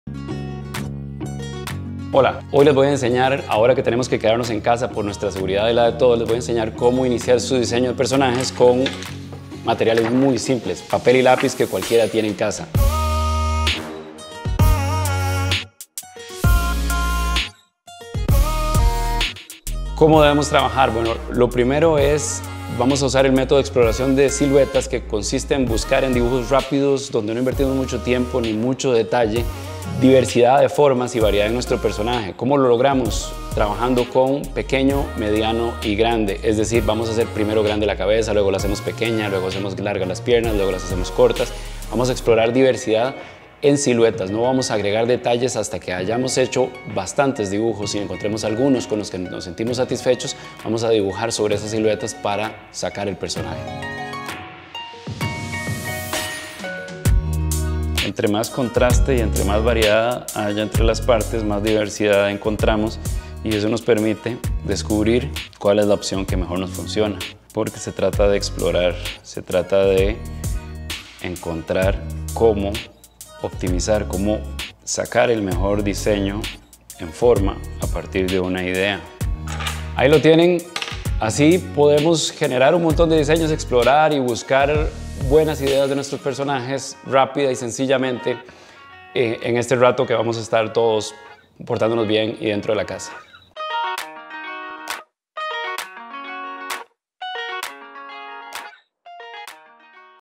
Consejos para la creación de personajes.
Douglas Campos profesor de Animación Digital en Universidad Veritas nos da una serie de consejos para la creación de nuestro dibujo de personajes